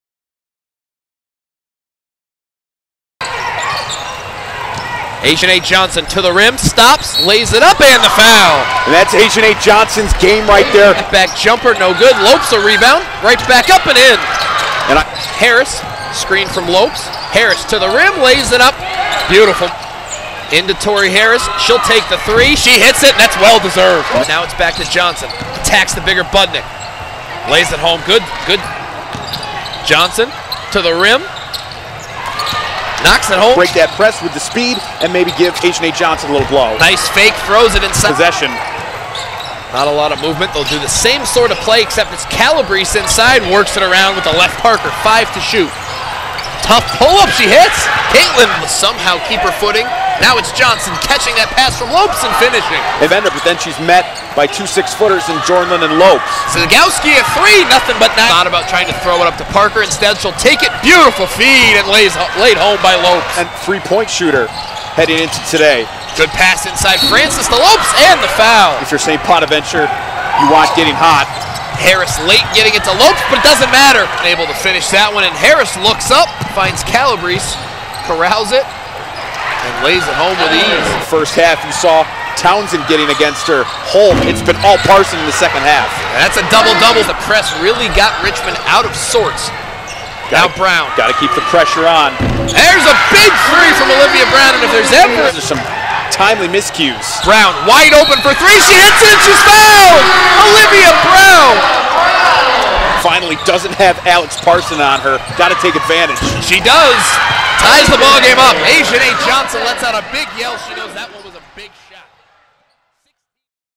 WBB | St. Bonaventure vs. Richmond (Jan. 8, 2021)
The St. Bonaventure Bonnies women's basketball team came up short Friday against the Richmond Spiders, 59-58.

Asianae Johnson led the way for the Bonnies with 18 points while Tori Harris chipped in 12 points and 11 rebounds.